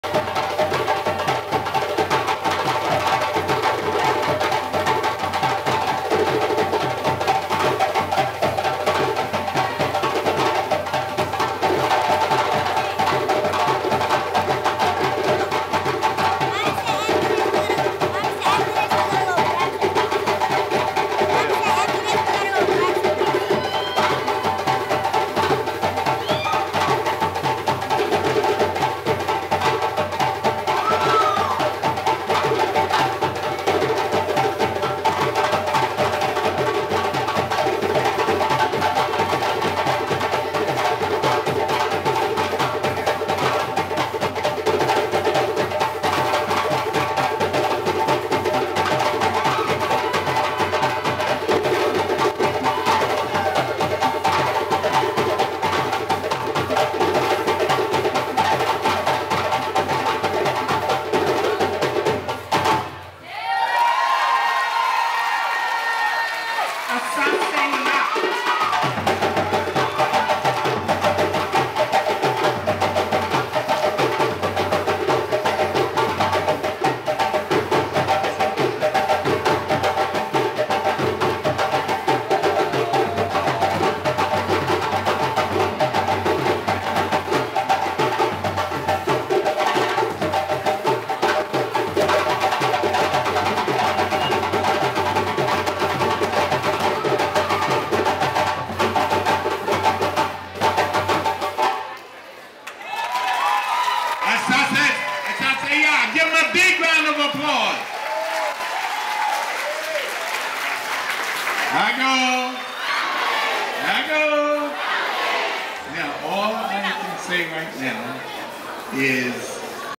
ASASE YAA performs during BAM'S PRE DANCE AFRICA SHOW
Asase Yaa Entertainment Group LLC.
160 Parkside Avenue, Suite # 12C

ASASE YAA performs during the 2011 BAM Pre Dance Africa event at Restoration Plaza 5/21/11.  Asase-Yaa will not be performing during this years Dance Africa Festival.  We only took part in the pre-show event by closing the days festivities as requested of Baba Chuck Davis founder & Artistic Director of Dance Africa/Dance Africa America.